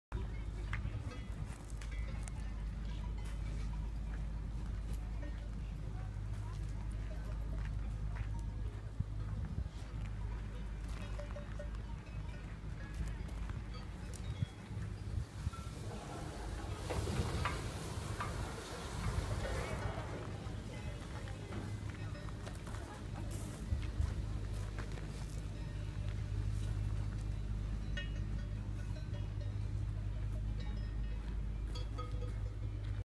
スイス発 Fronalpstock(1922m)からの絶景【スイス情報.com】
リフトでFronalpstock(1922m)へ。山頂から眺めるアルプスの山々とルツェルン湖やツーク湖を始めとする湖の景色が素晴らしいです。

Swissjoho.com

Y.M.